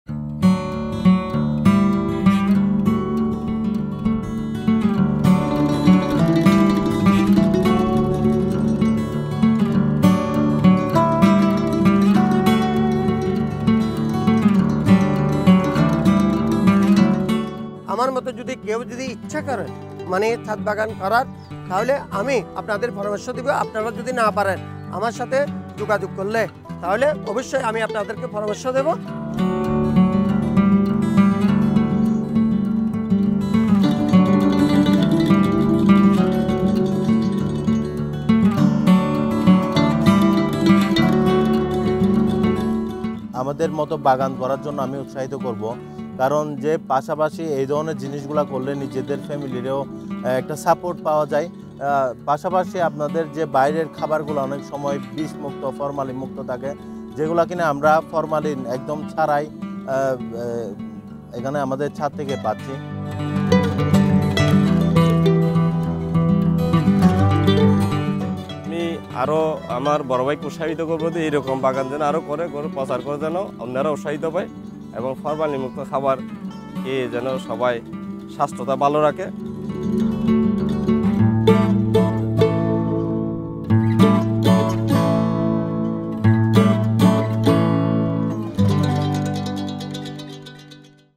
২৪ বছর ধরে ছাদবাগান করছেন যিনি | Rooftop Garden
২৪ বছর ধরে ছাদবাগান করছেন যিনি | Rooftop Garden

সবজি দিয়ে ছাদবাগান শুরু করেছিলেন জ্বালানি ব্যবসায়ী নুরুল ইসলাম। আর এখন ফুলে-ফসলে পাল্টে গেছে তাঁর বাড়ির চেহারাটাই